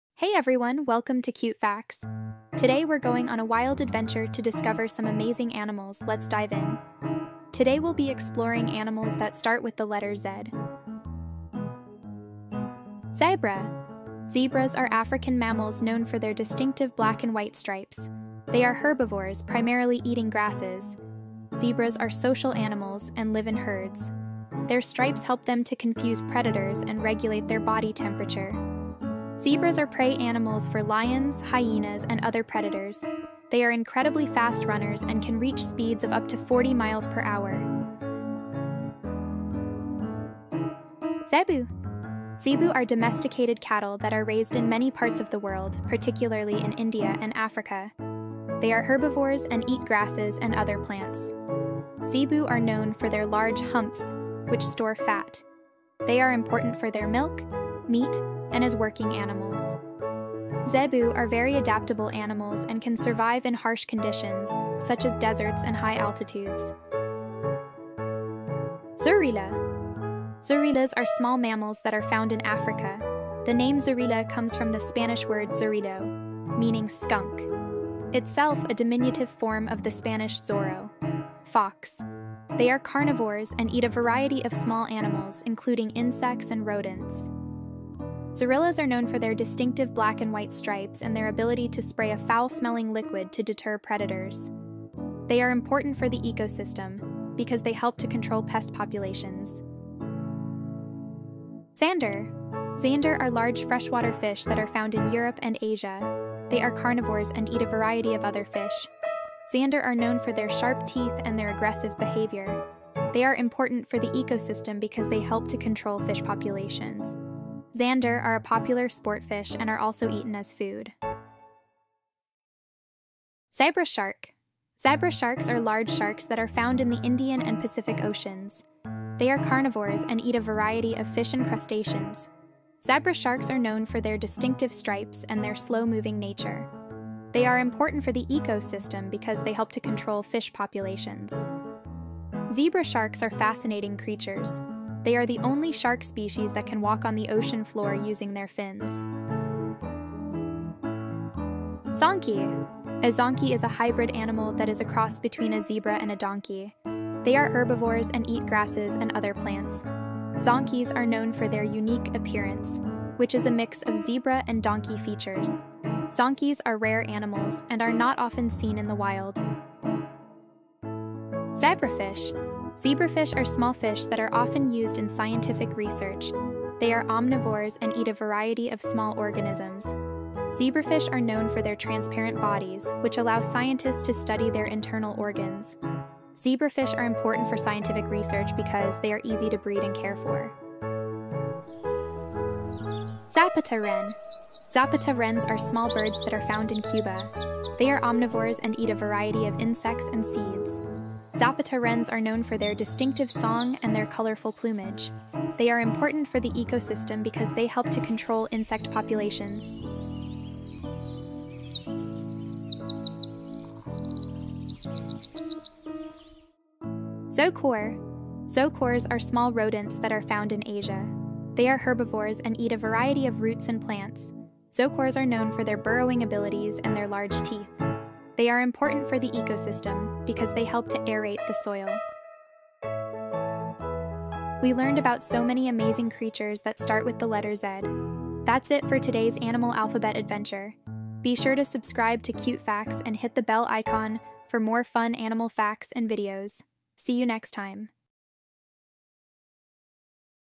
Discover Awesome Animals That Start With Z For Cute Little Adventurers! | Cute Facts
Dive into the world of animals starting with the letter Z. From the striped zebra to the playful zorilla, explore their unique habitats, behaviors, and incredible abilities. This educational video is perfect for curious kids who love learning about wildlife.

1. Zebra
2. Zebu
3. Zorilla
4. Zander
5. Zebra Shark
6. Zonkey
7. Zebrafish
8. Zapata Wren
9. Zokor